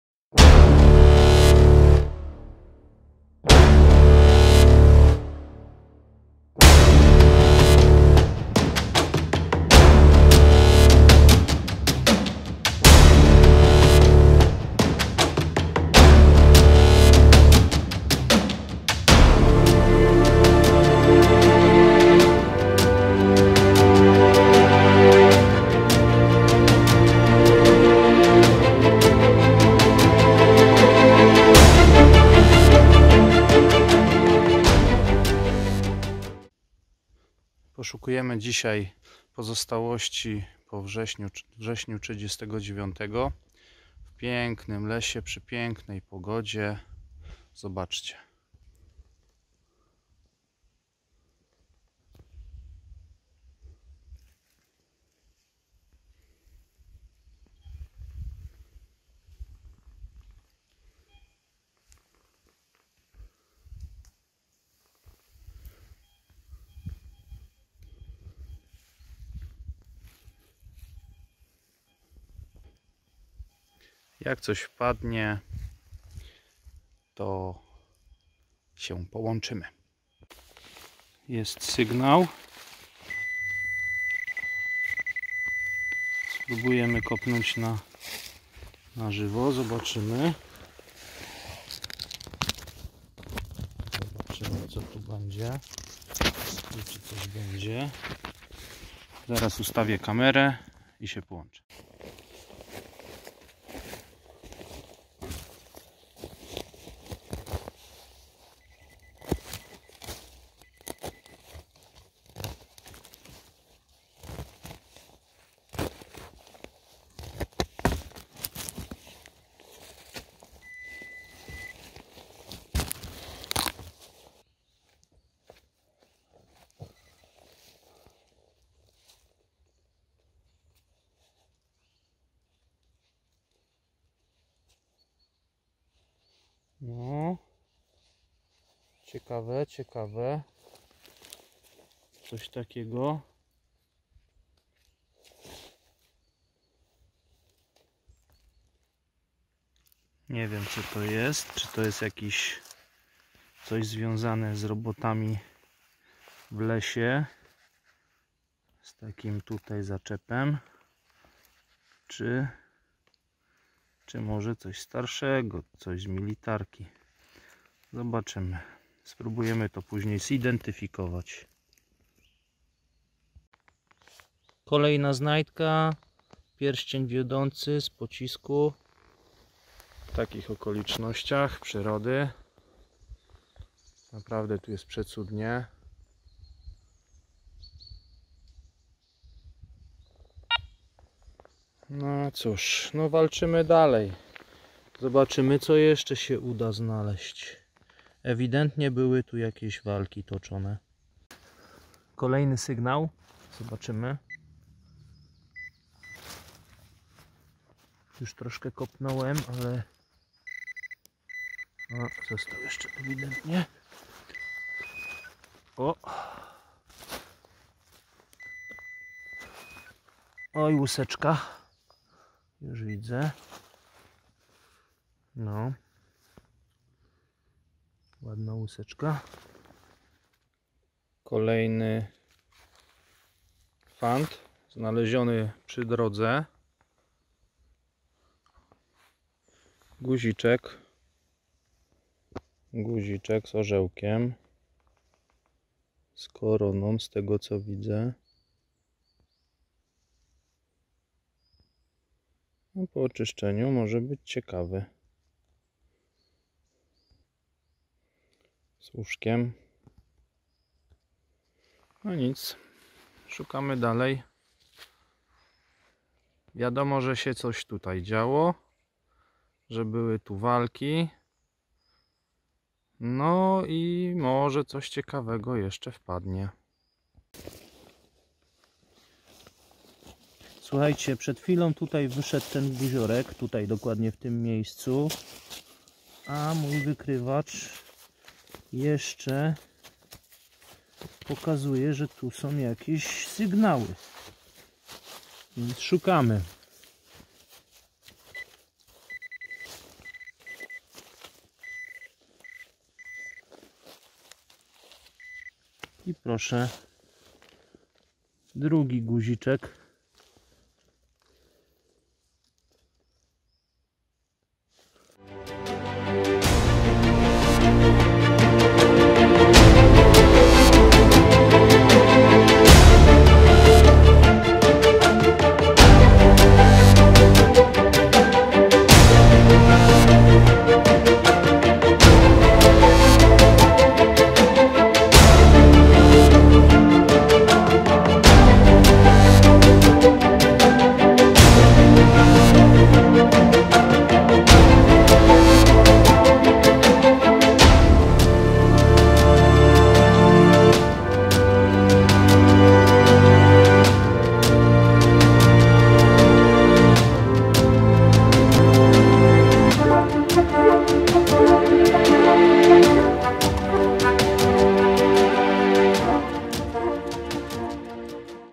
Złoty Potok
Poszukiwania pozostałości po 1939r. w Złotym Potoku.